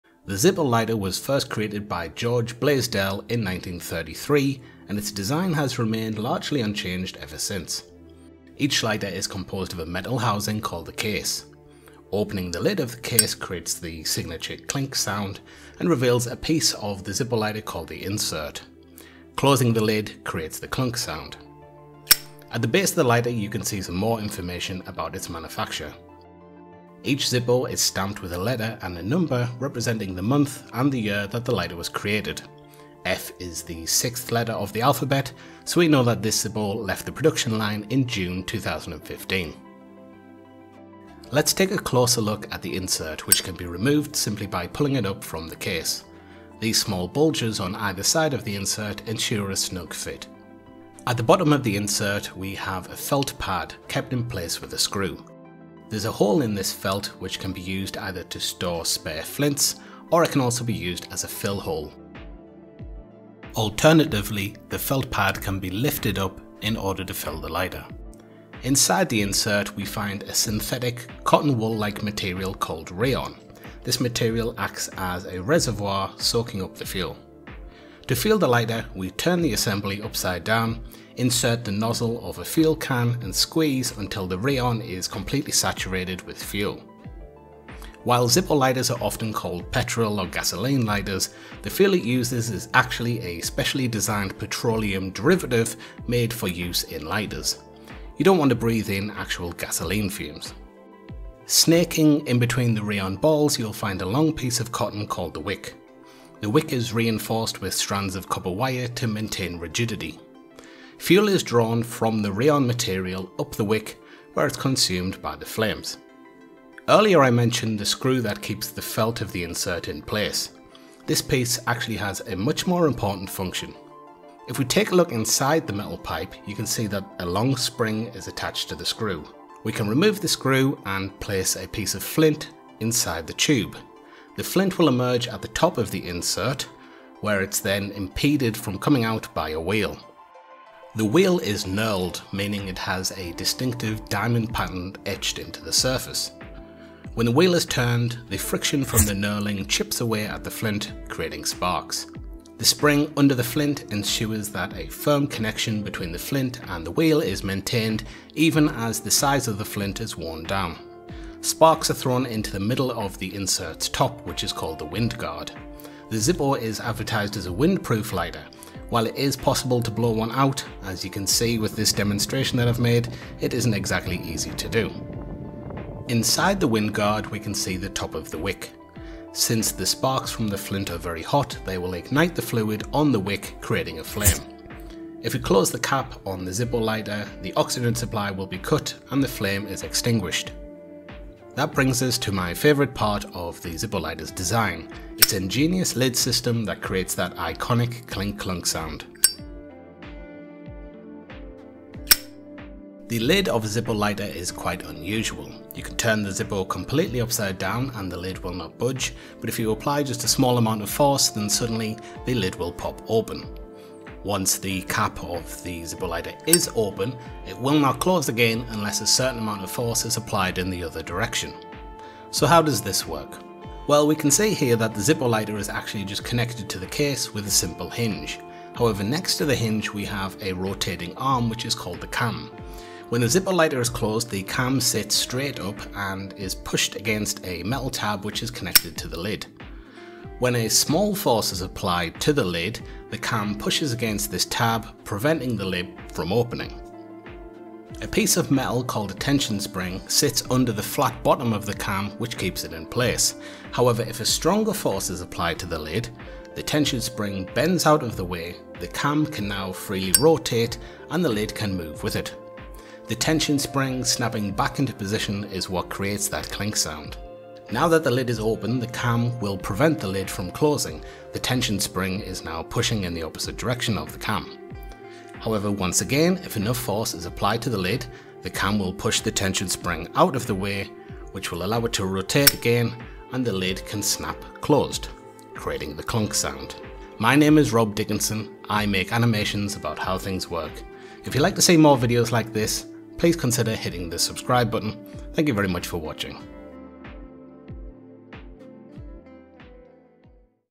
How a Zippo Lighter Works
The Zippo lighter is a true icon of 20th century design. Largely unchanged since it's introduction, the Zippo lighter is a fascinating piece of engineering.

In this video, we'll take a look at all of the major components of a Zippo lighter.